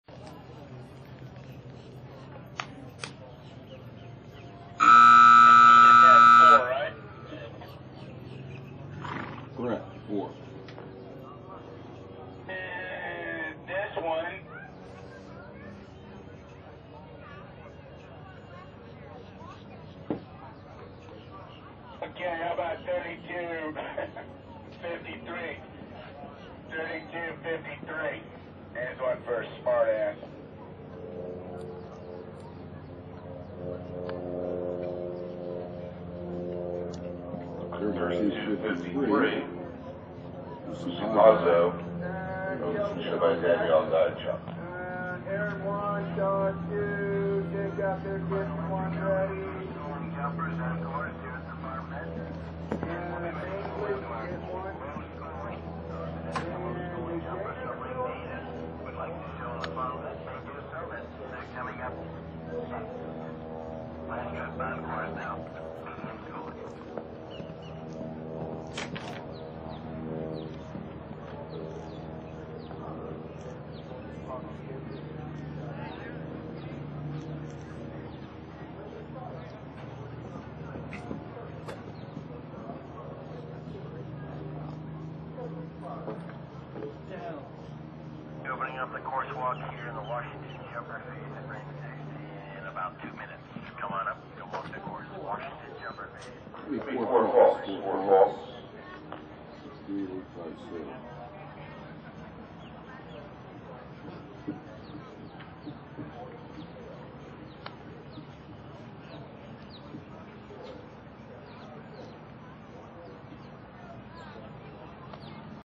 Video of TU SI PAZZO riden by DANELLE DIACHUK from ShowNet!
TU SI PAZZO riden by DANELLE DIACHUK at WEF VII.  This round is from class 1041, $1500 Adult Am 18-35 Time 1st Jump Off and finished 15 of 24.